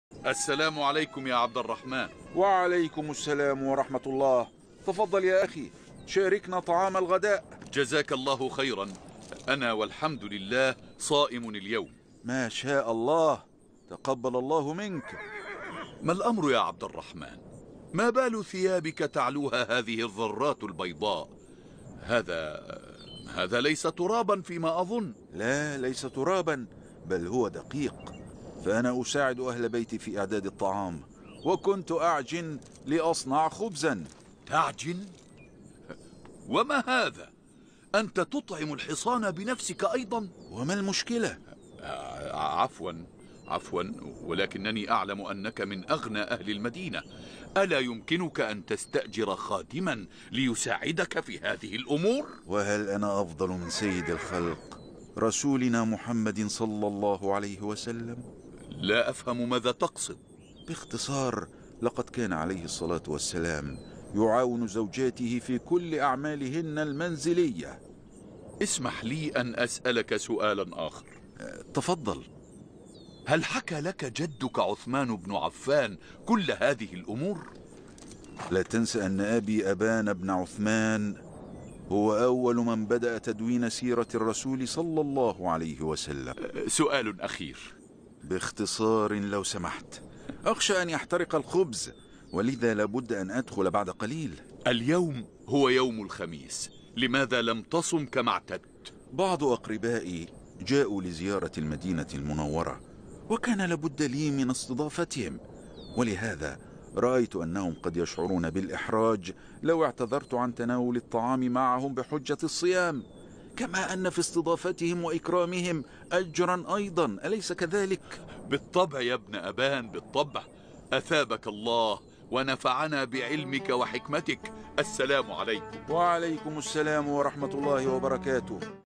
La modestie de Abdurrahman ibn Abaan - Parmi les histoires des Tābi'in
La modestie du grand Tābi'i et savant Abdurrahman ibn Abaan, le petit fils du compagnon Othmane ibn Affân -qu'Allah l'agrée-.
Un extrait de votre nouvelle série sur les histoires des Tābi'in (personnes ayant vécu à l'époque des compagnons).